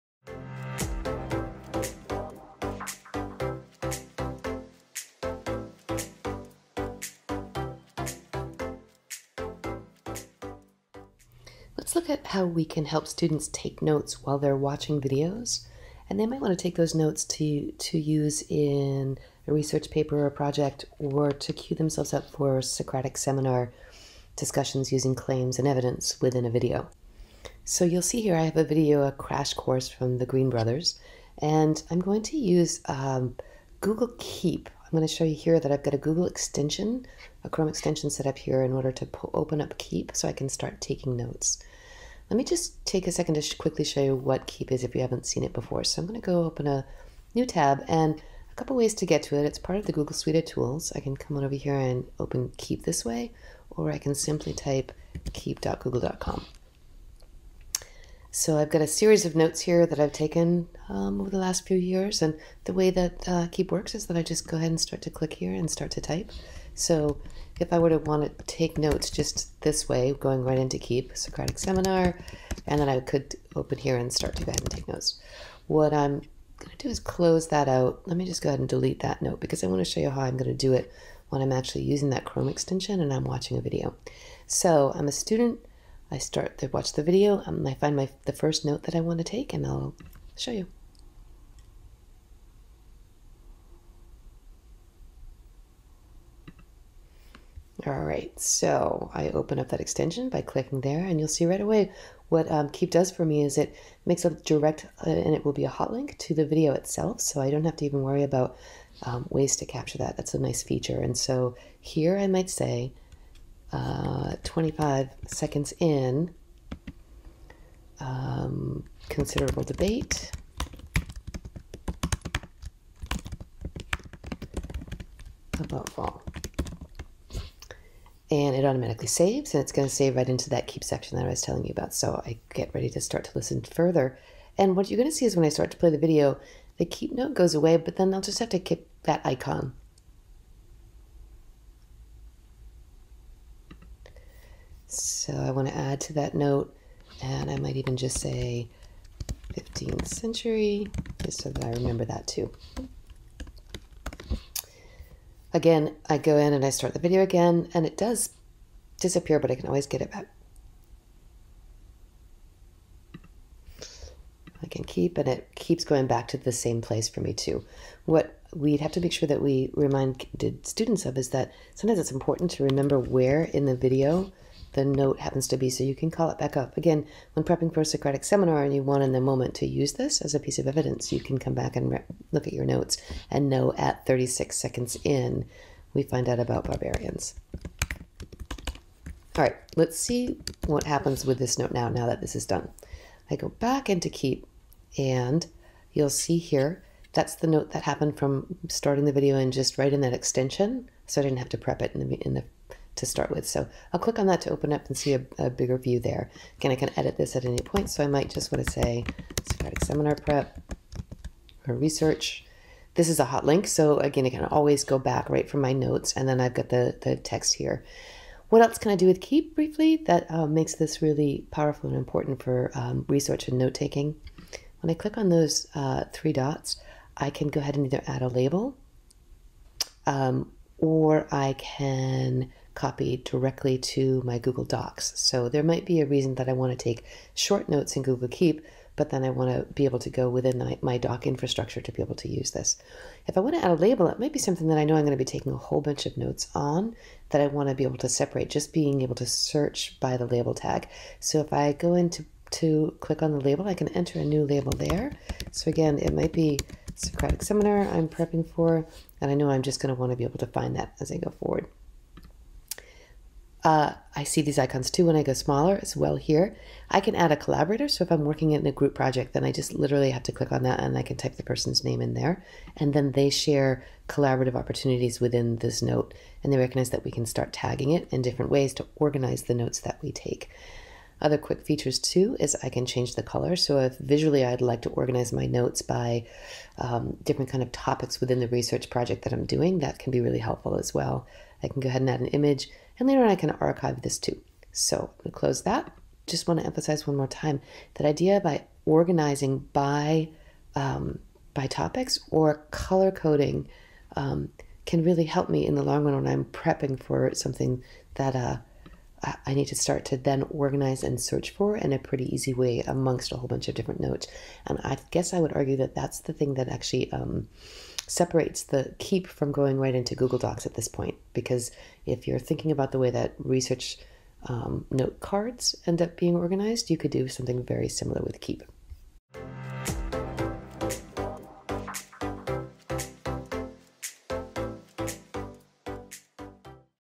How to use Google Keep for research notes
In another installment of our Tech Tips for Teachers, Vermont educator Susan Hennessey shows you how students can use the Google Keep browser add-on in Chrome to support taking notes during video-watching. This can be an invaluable resource for students who consult videos as they do research, and help them learn how to curate their notes and self-manage the process.